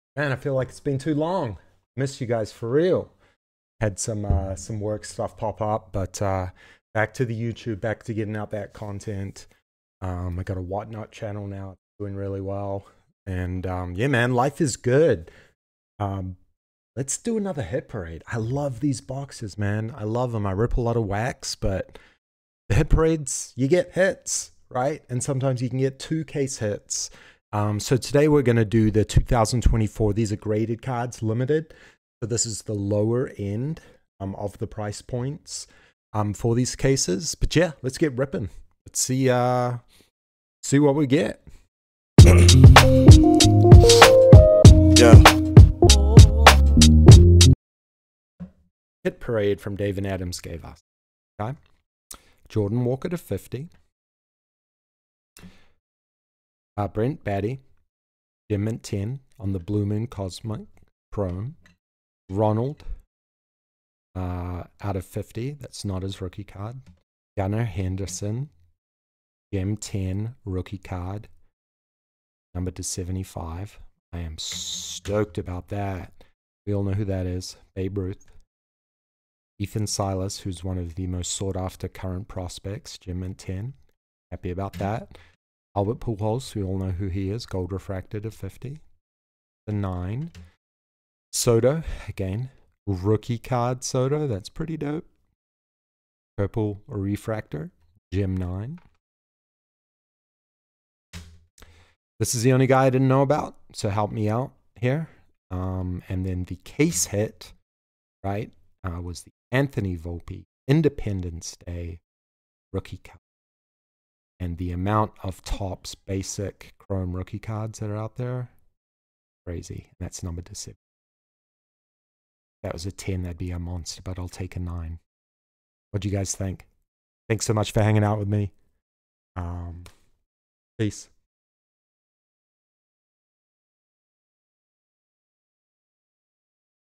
Excitement of the MLB Graded Hit Parade - Full Case🔥
⚾️ Featuring top cards from players like Gunnar Henderson, Anthony Volpe, and Juan Soto. Baseball fans, don't miss this epic collection! 🔥 MLB ⚾️ @dacardworld

Whatnot - reckscollects
Sportbetting - Recks_Collects on the MyAction App